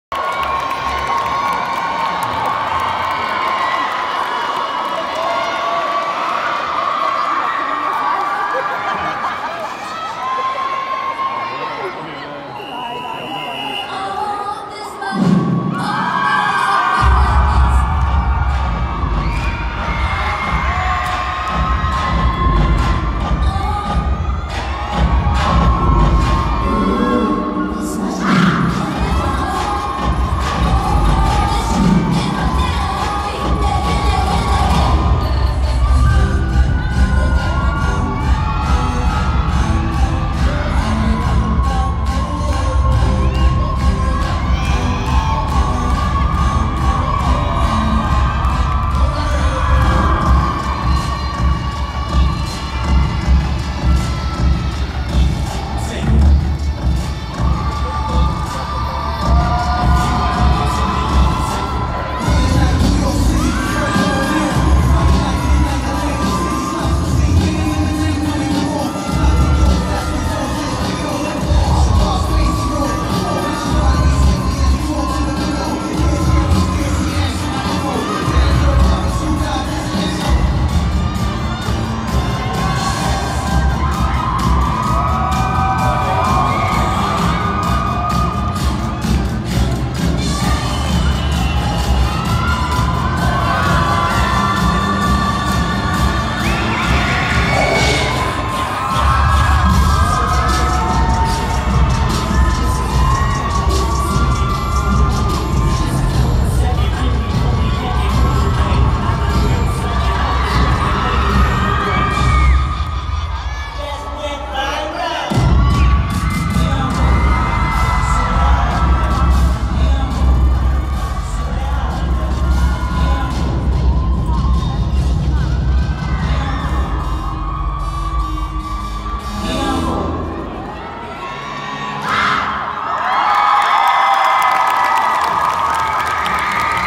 KIF Studio3 ZeeCrew | ECU European Championship 2019 - Semi Finals
7th place (311.5p), Junior Hip Hop

NORWAY